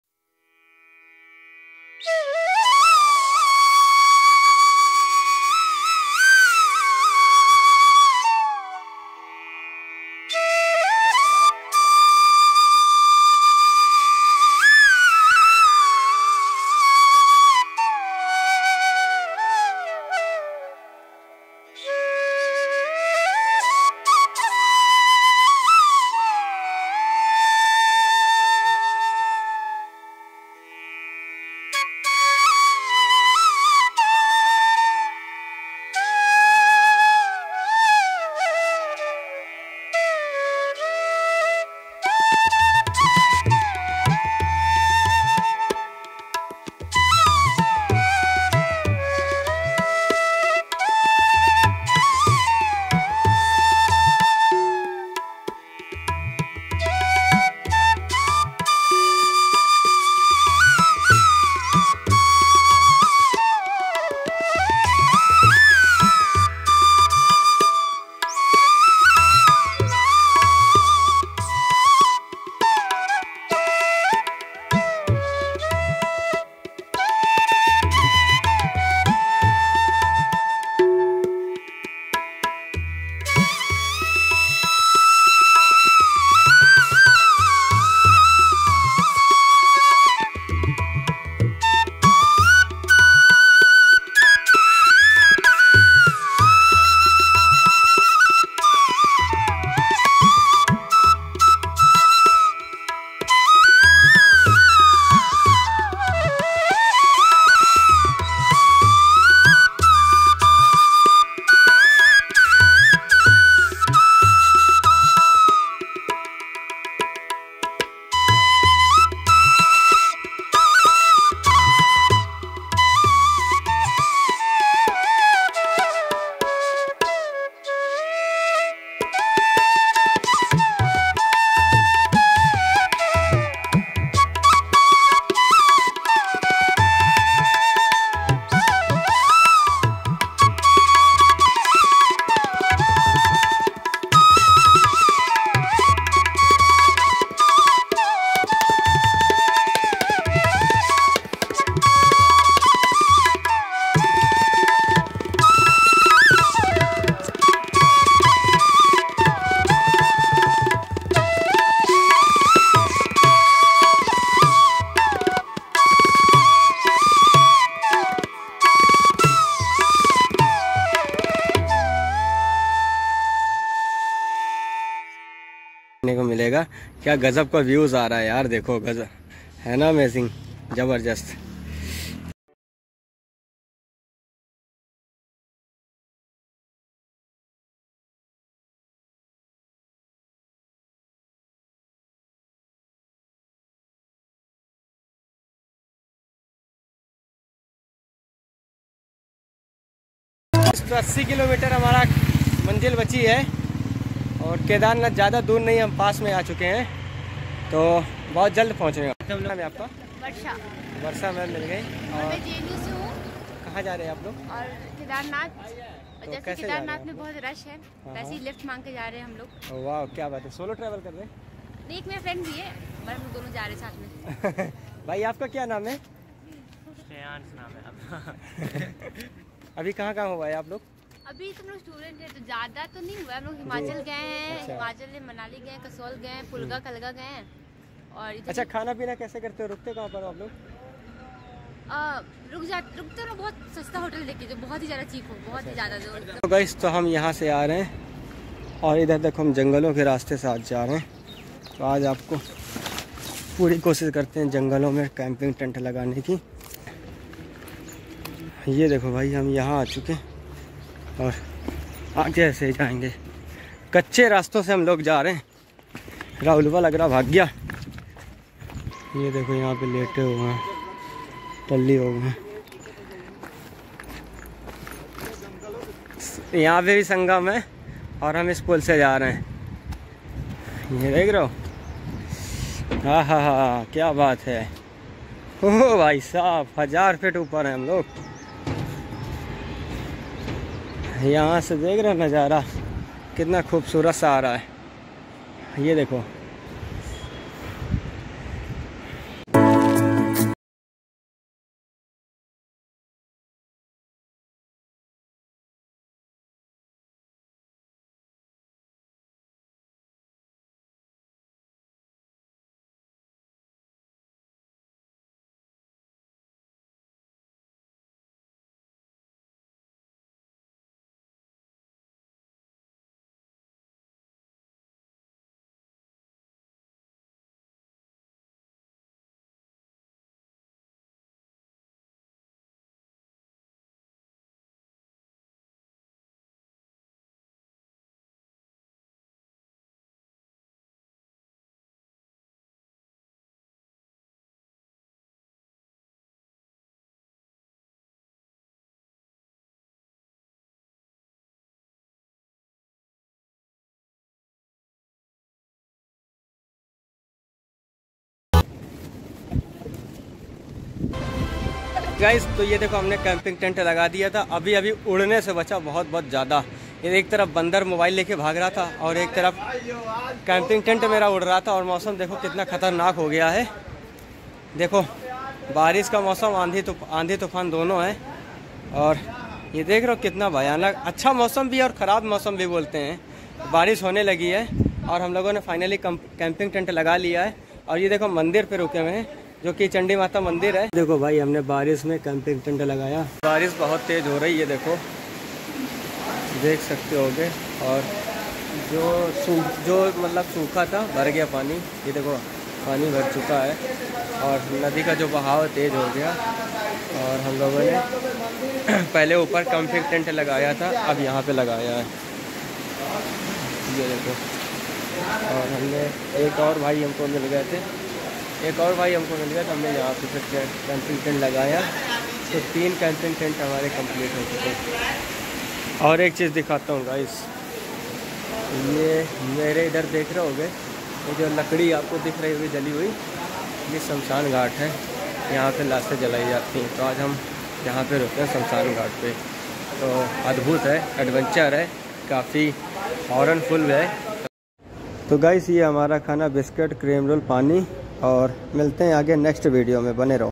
Raste mei mila surprise 🤩🤩
Raste mei mili cute girl 🤩 boli hme bhi le chalo